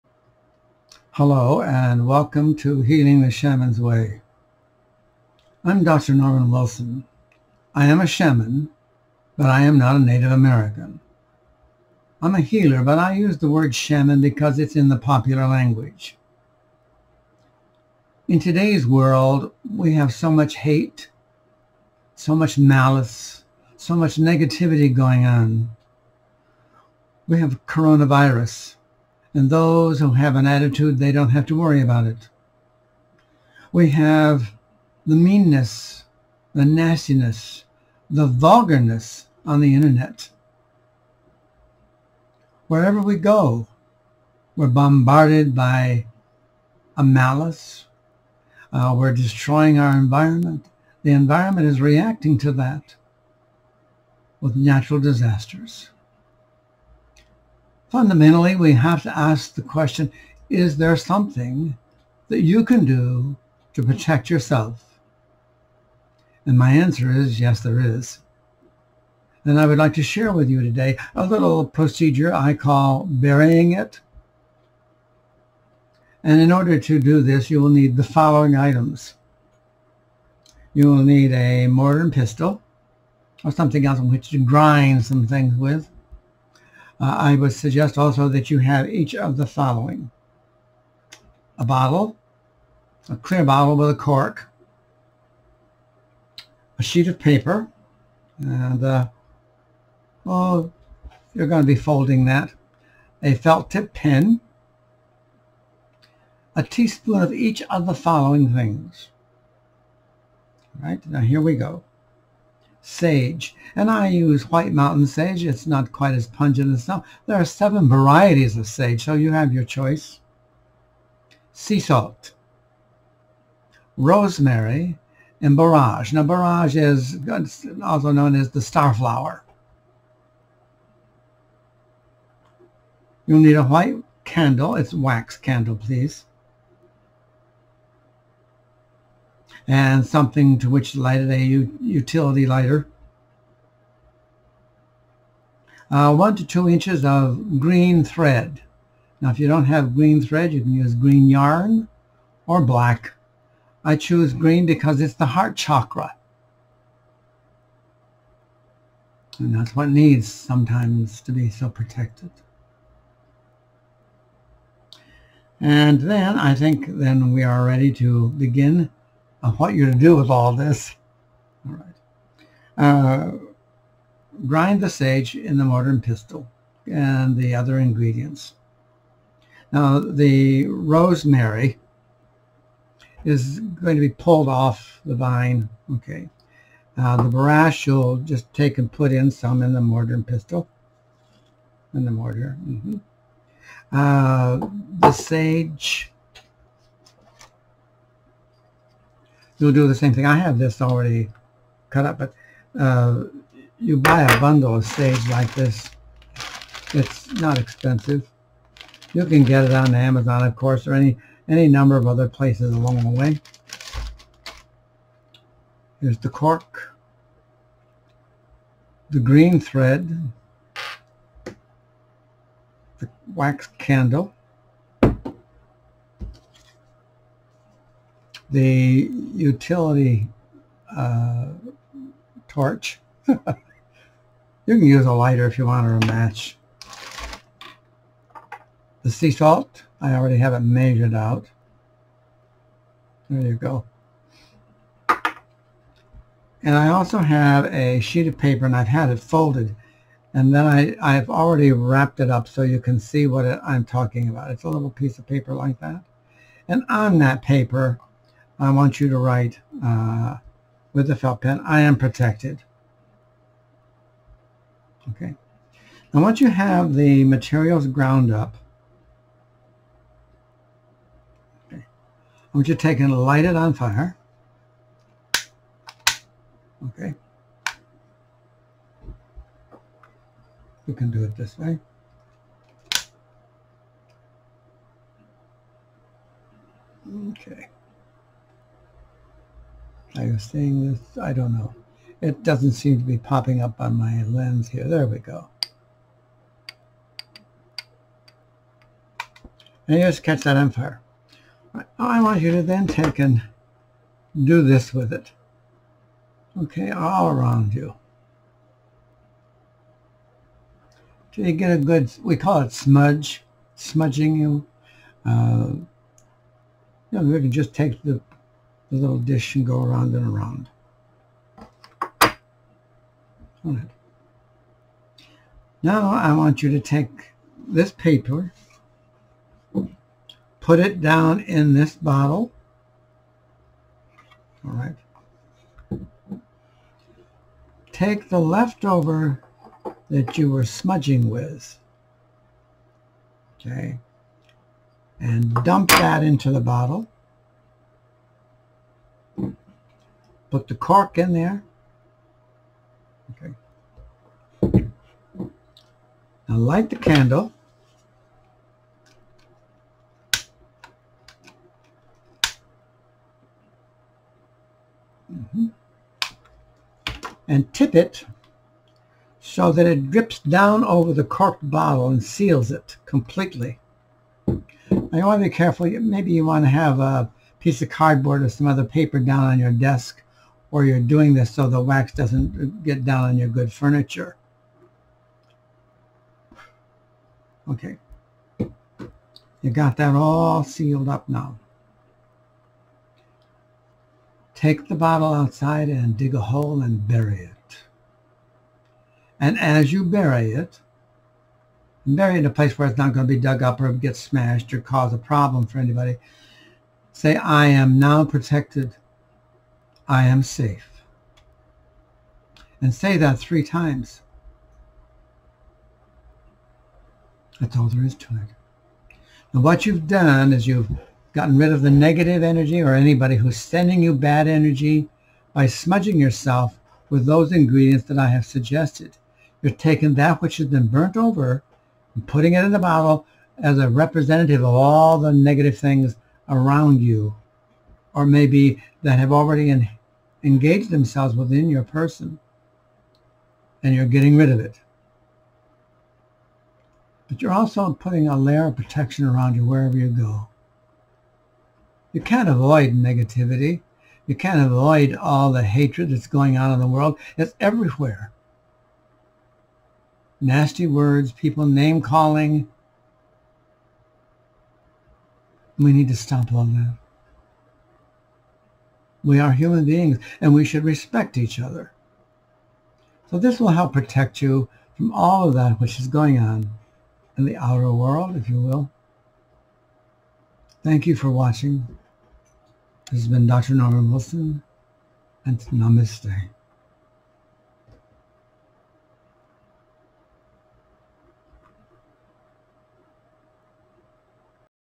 Protect Yourself the Shamans Way with Dr. Norman Wilson
at 7pm for a streaming broadcast of Dr. Norman Wilson guiding viewers through conducting a personal protection ritual. The recording will be available to patrons

The Talk Story Today Show features interviews and talks by shamans, wisdom teachers, healers, and artists.